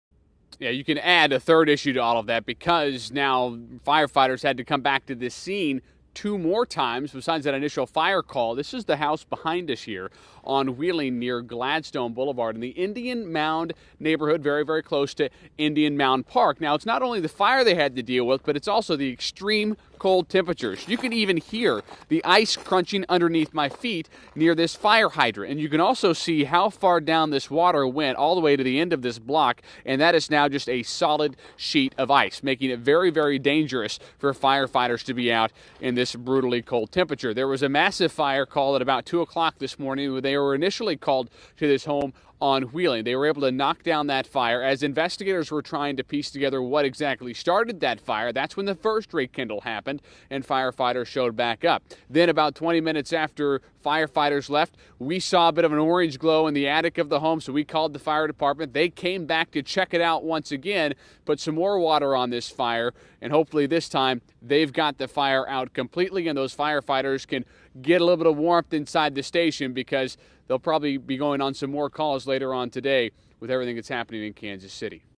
KC firefighters battle house fire early Tuesday on city's northeast side
Kansas City firefighters battled a house fire early Tuesday on the city's northeast side. No injuries were reported.